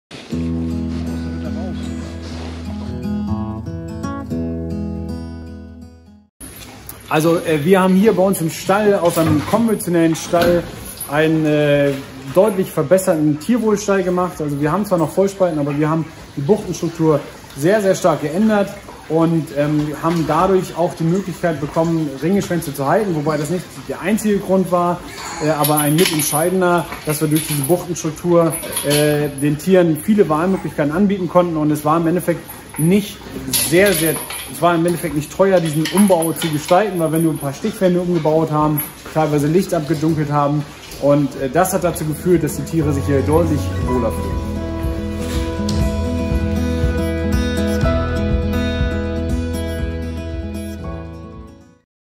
Neues Strukturkonzept für die Schweinemast in Altbauten
Christoph Becker (Modell- und Demonstrationsvorhaben Tierschutz - Betrieb) erzählt uns, wie er seinen Altbau umgestaltet und dadurch eine verbesserte Haltungsumgebung für seine Schweine geschaffen hat.